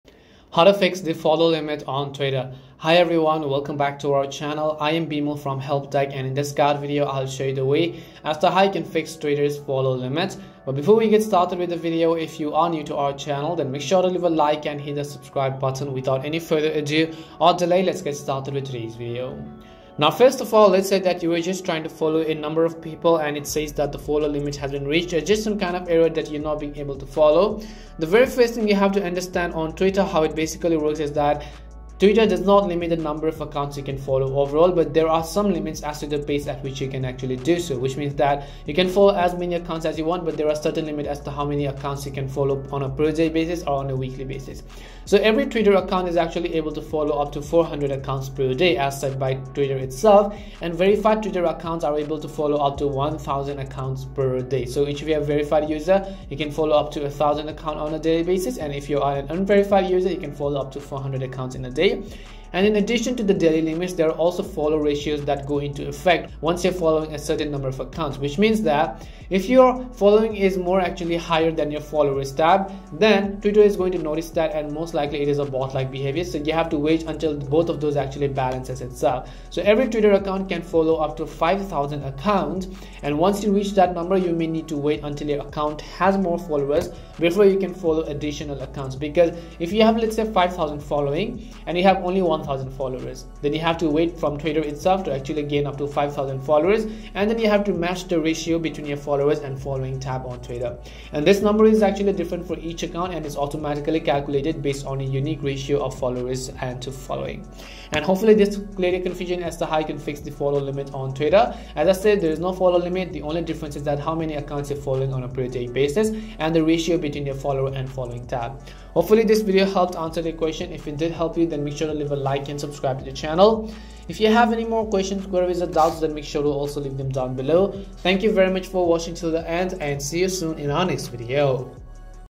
How to Fix Twitter Follow Limit !
This video guides you in quick easy steps to fix the follow limit on Twitter. So make sure to watch this video till the end.

The limit cannot be overcome and your account's limit cannot be lifted by Twitter's help desk. Waiting until your account has more followers will alter the ratio and relax the account limit, making it possible to follow people again.

~ Time Stamps:
0:00 Introduction
0:20 How to Fix Twitter Follow Limit
1:48 Outro: Final Verdict